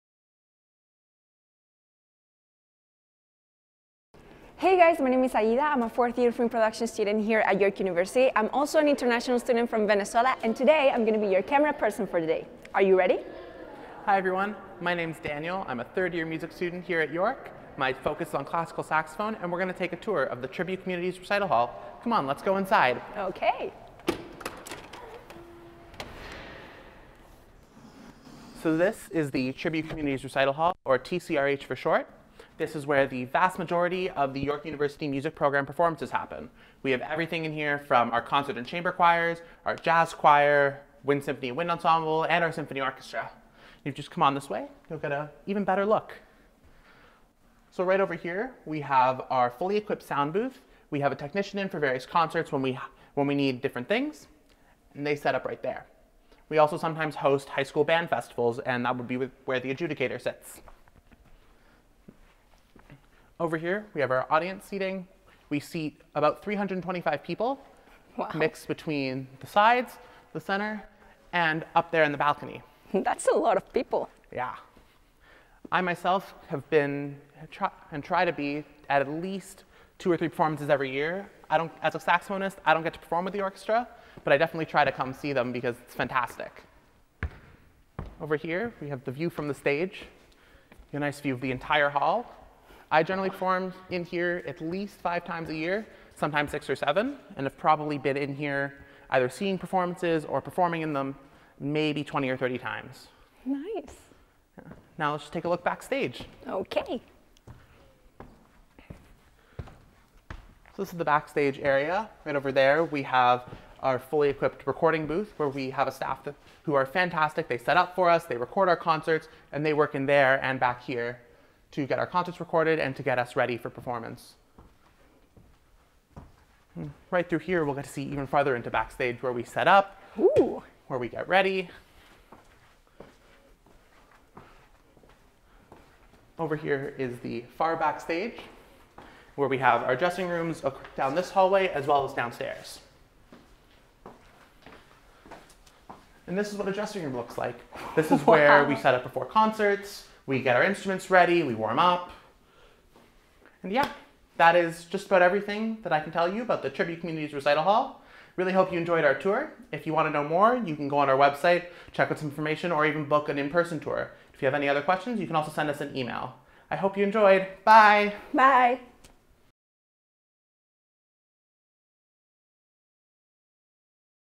360° Tour - Tribute Communities Recital Hall
The Tribute Communities Recital Hall is an intimate, 320-seat concert venue with an integrated recording studio. It is ideal for a wide range of music presentations including recitals, orchestral, chamber and choral concerts, in classical, jazz and world music. Acoustically designed by Artec, the hall can be tuned via adjustable acoustic banners to optimize the sound of various instruments, configurations and styles of performance, and has a sound system that accommodates lightly amplified performances. Facilities include a grand piano, flexible riser system and ample dressing rooms.